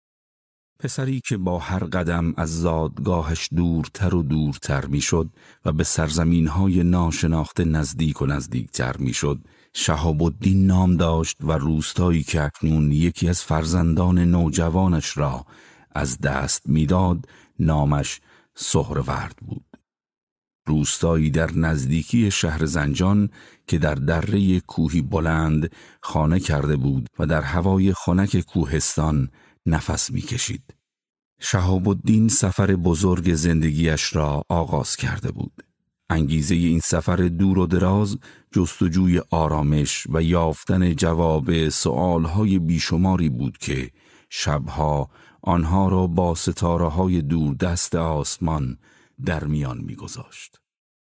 پادکست: کتاب صوتی سهروردی
کتاب صوتی سهروردی اثر مسعود علیا با صدای بهروز طالبی. با مراجعه به وبسایت یا اپلیکیشن آوانامه از طریق لینک زیر نسخه کامل این کتاب صوتی را دانلود کنید